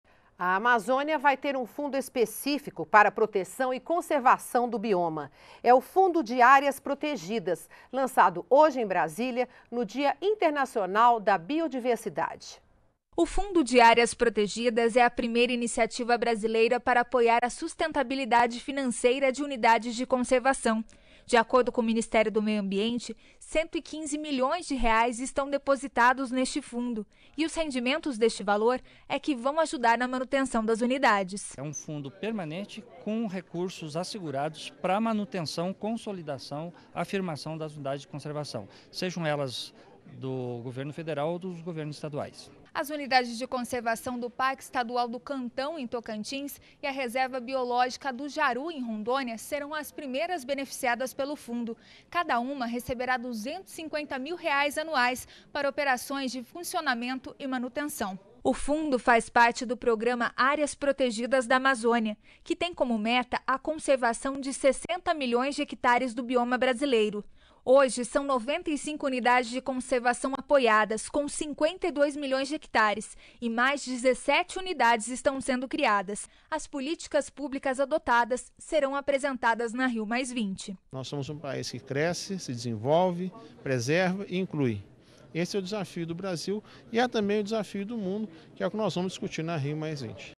Amazônia ganha fundo específico para proteção e conservação do bioma
NBR NOTÍCIAS - 22.05.12: Nesta terça-feira (22), dia internacional da biodiversidade, foi lançado o Fundo de Áreas Protegidas. A iniciativa é pioneira no Brasil e vai apoiar a sustentabilidade financeira de unidades de conservação da Amazônia. De acordo com o Ministério do Meio Ambiente, os recursos do fundo somam R$115 milhões. Atualmente, existem 95 pontos de conservação apoiadas com 52 milhões de hectares. Outras 17 unidades, que estão sendo criadas com o fundo, serão apresentadas durante a Conferência das Nações Unidades para o Desenvolvimento Sustentável, a Rio +20